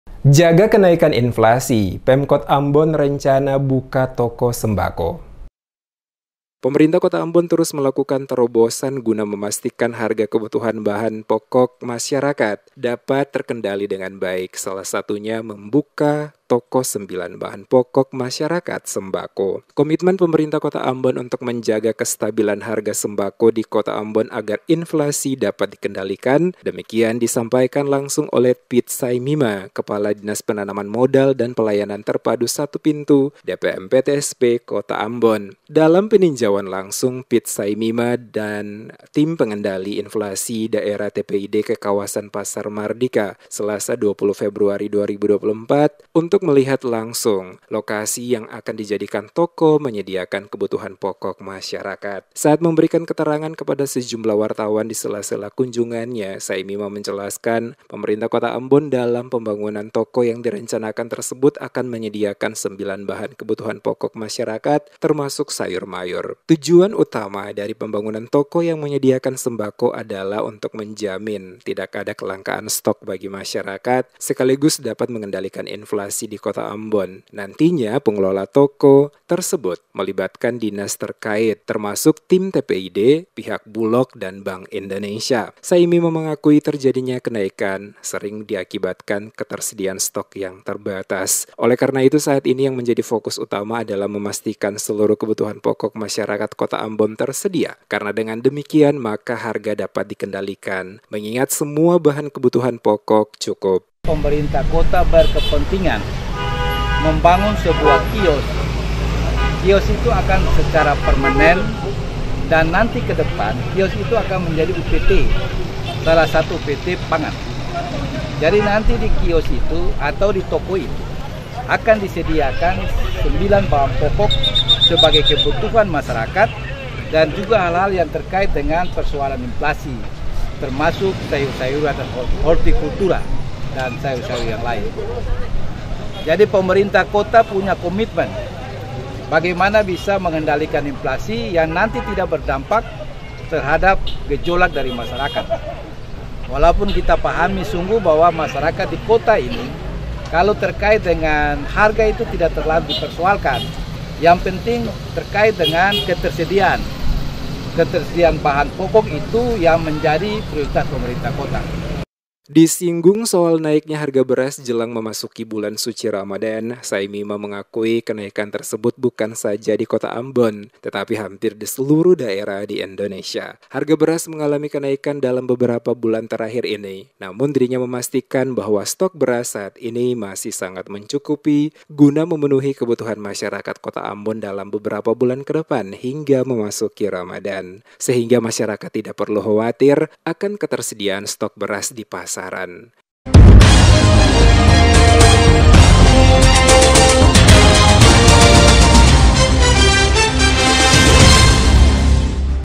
Jaga Kenaikan Inflasi, Pemkot Ambon Rencana Buka Toko Sembako
Berita Ambon – Pemerintah Kota Ambon terus melakukan terobosan guna memastikan harga kebutuhan pokok masyarakat dapat terkendali dengan baik, salah satunya dengan membuka toko Sembilan bahan pokok masyarakat (Sembako).